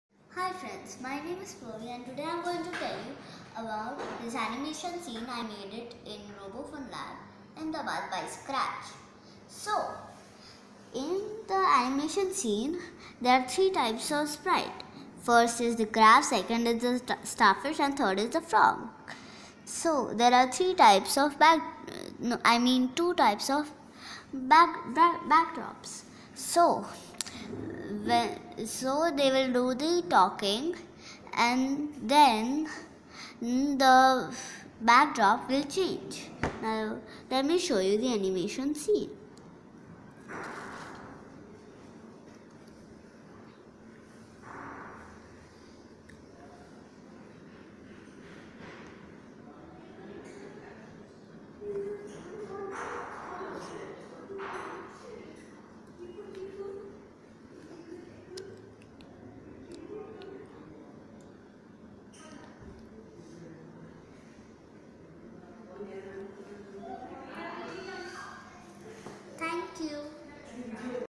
Animation Story - Pavi Jani - RFL Ahmedabad (8980172306)
Animation Story using Scratch programming. it programmed By : Pavi Jani